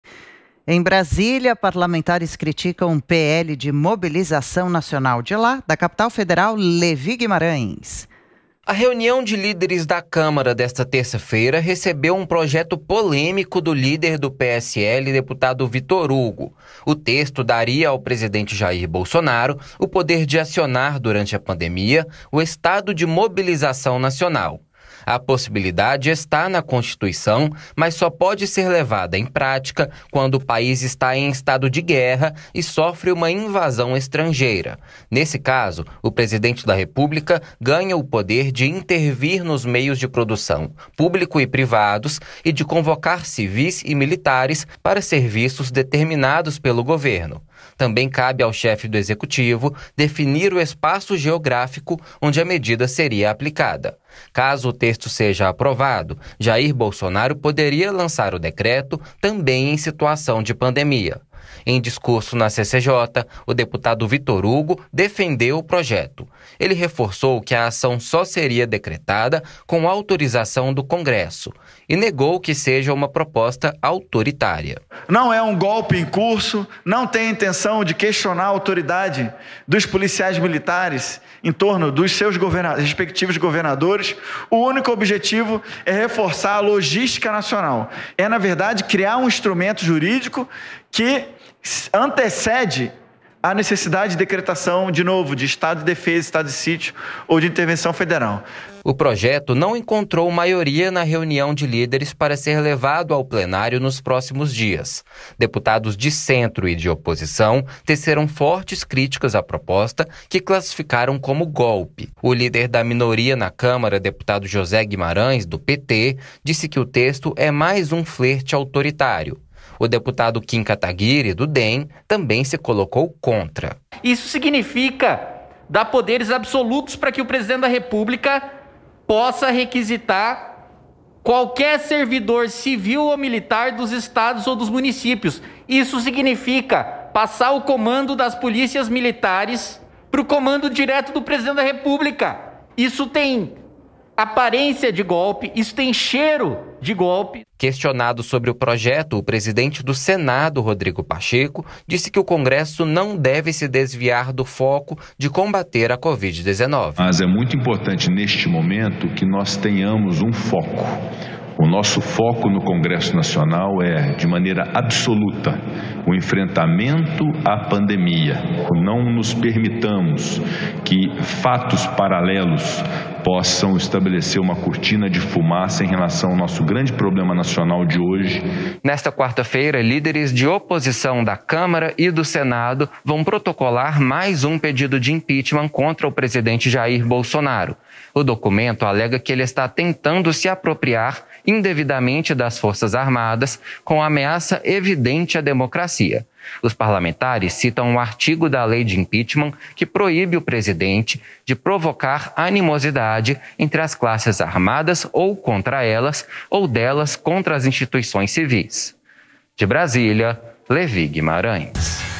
Oposição quer impeachment por ação de Bolsonaro nas forças armadas
A reunião de líderes da Câmara desta terça-feira (30) recebeu um projeto polêmico do líder do PSL, deputado Vitor Hugo. O texto daria ao presidente Bolsonaro o poder de acionar o estado de mobilização nacional durante a pandemia. A possibilidade está na Constituição mas só pode ser levada em prática quando o país está em guerra e sofre invasão estrangeira. Neste caso, o presidente da República ganha o poder de intervir nos meios de produção e convocar civis e militares. Parlamentares de centro e oposição classificam proposta como golpe.